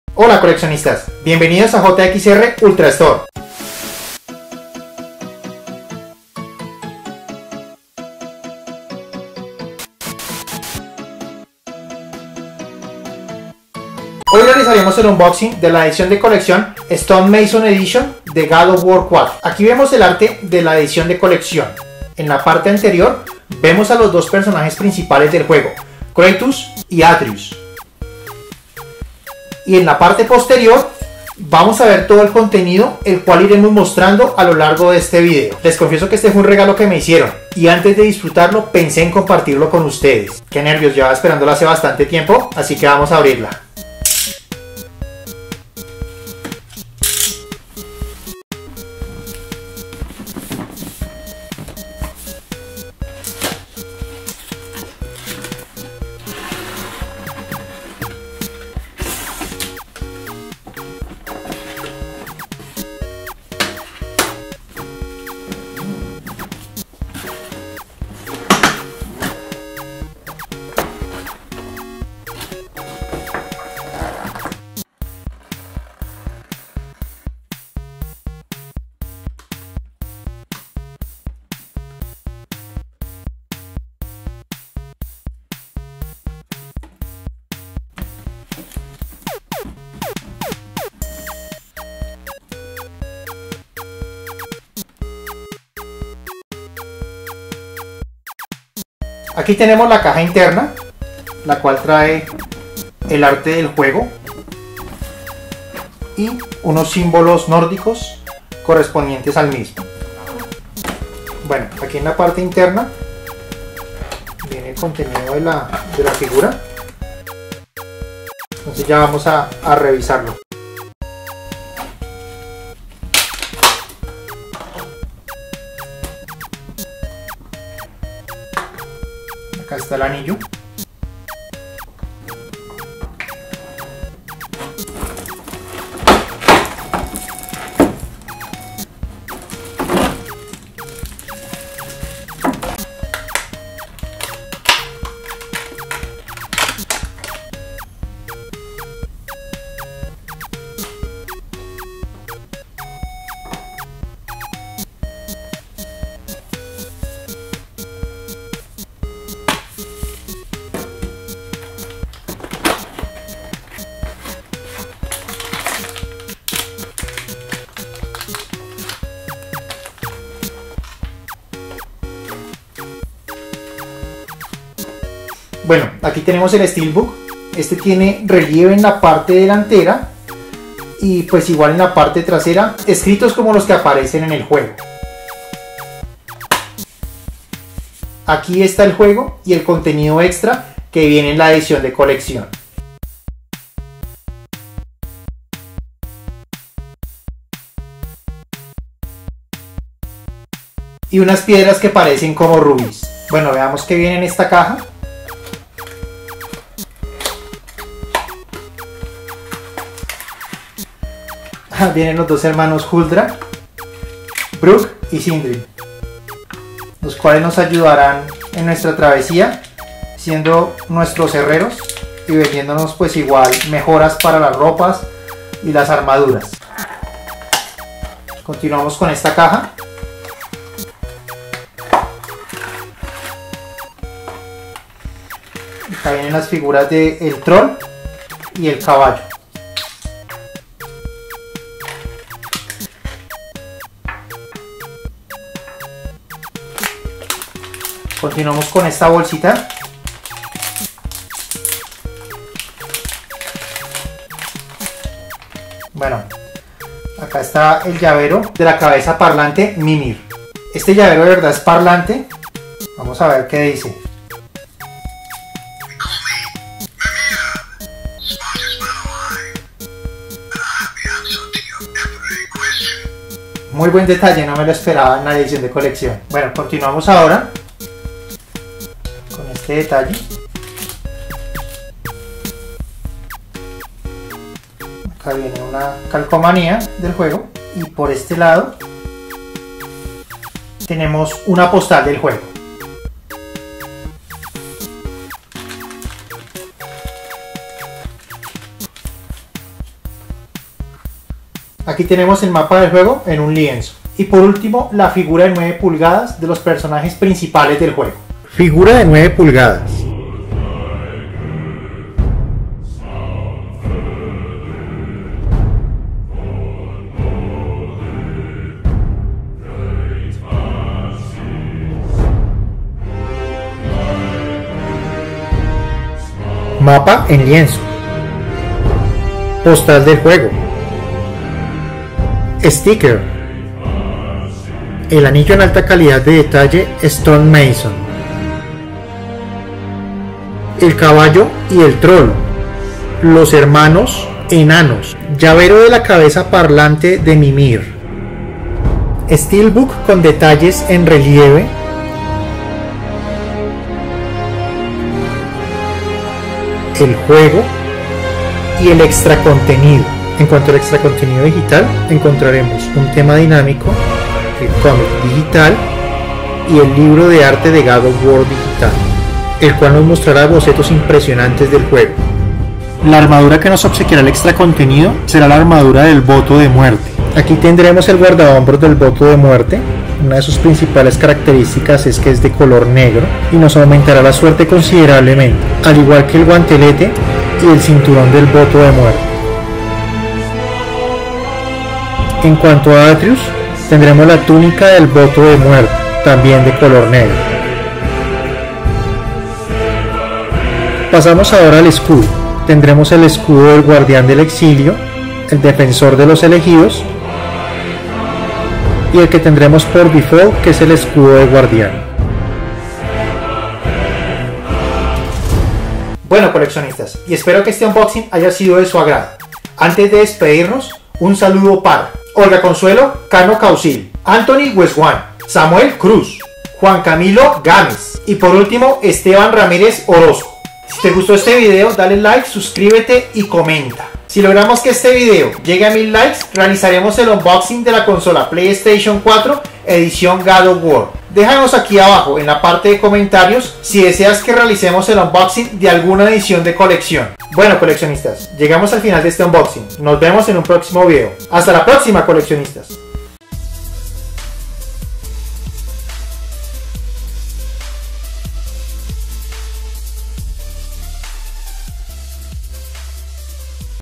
God Of War 4 Edición Coleccionista PlayStation 4 Unboxing Review Colombia - JxR
Hola a todos el video del día de hoy enseñaremos la edición de colección de god of war ¡Disfrutala!.

Saludos y estaremos en contacto.